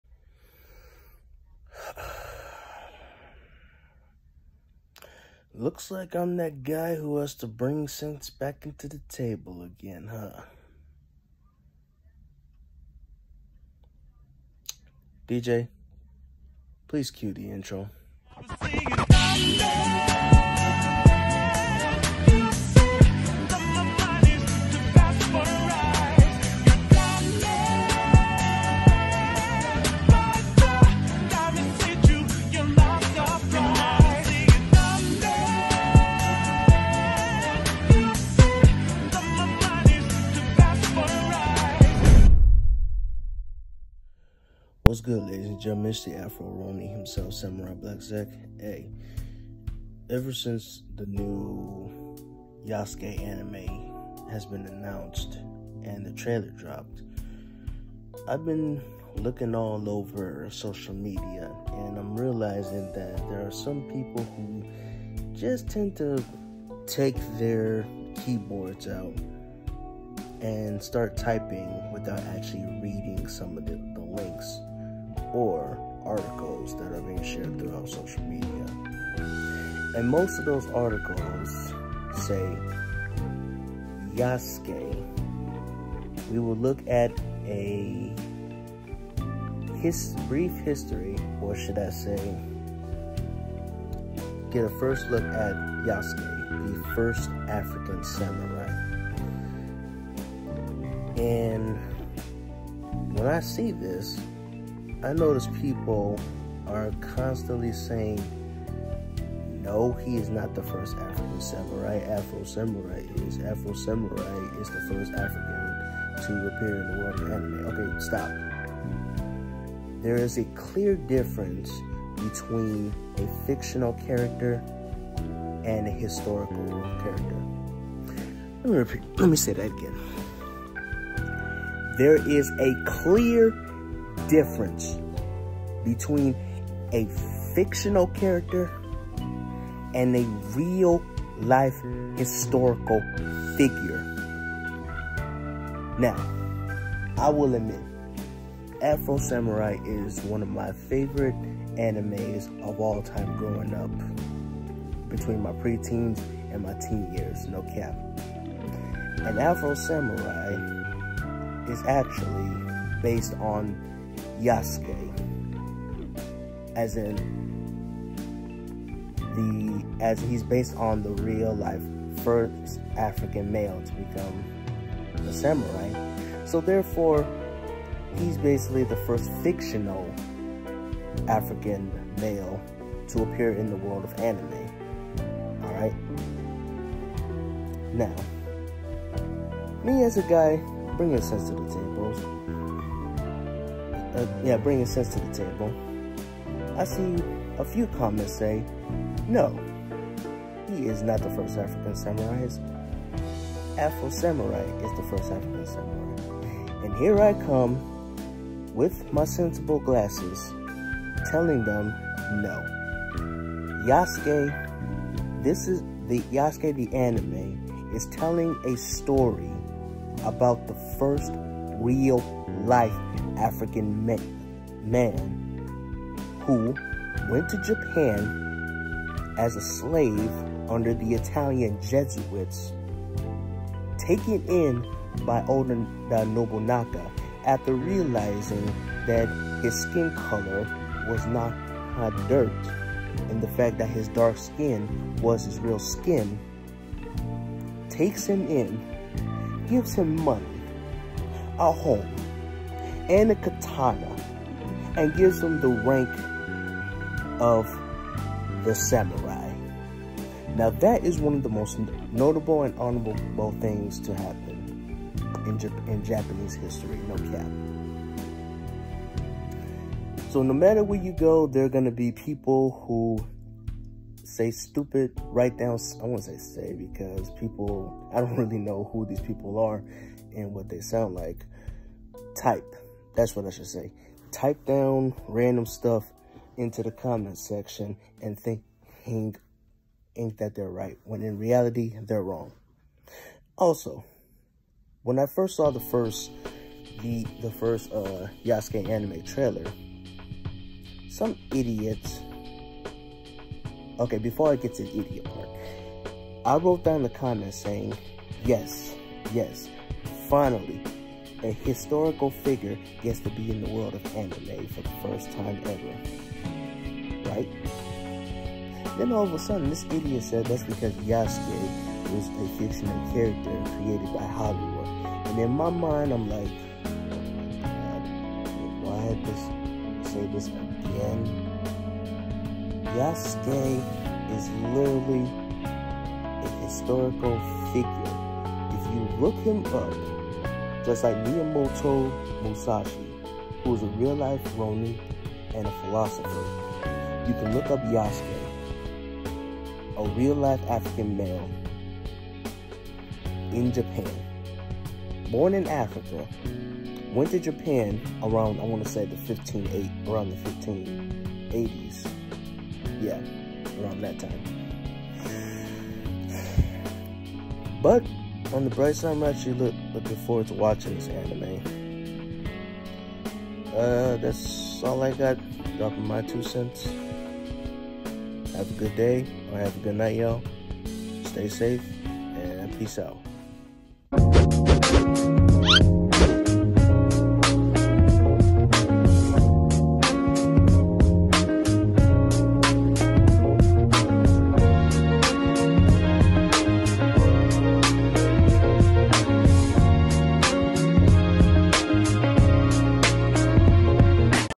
SBZ Address the Issues with the Yasuke hate train
The Yasuke anime is lit. Even tho it has six episodes its still awesome. But when people start saying stupid stuff without doing actual research on it is something i cannot tolerate.